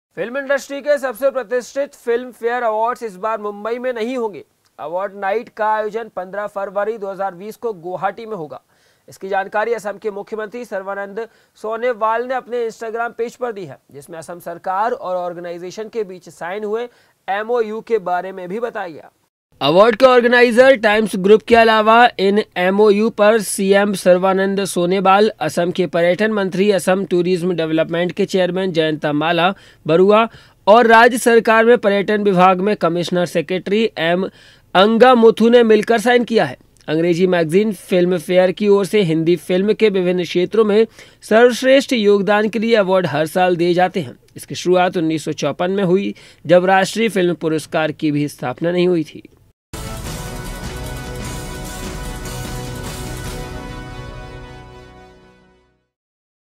MUMBAI 65वें फिल्म फेयर अवाॅर्ड्स
फिल्म इंडस्ट्री के सबसे प्रतिष्ठित फिल्म फेयर अवाॅर्ड्स इस बार मुंबई में नहीं होंगे। अवाॅर्ड नाइट का आयोजन 15 फरवरी 2020 को गुवाहाटी में होगा। इसकी जानकारी असम के मुख्यमंत्री सर्बानंद सोनोवाल ने अपने इंस्टाग्राम पेज पर दी है। जिसमें असम सरकार और ऑर्गनाइजर्स के बीच साइन हुए एमओयू के बारे में भी बताया गया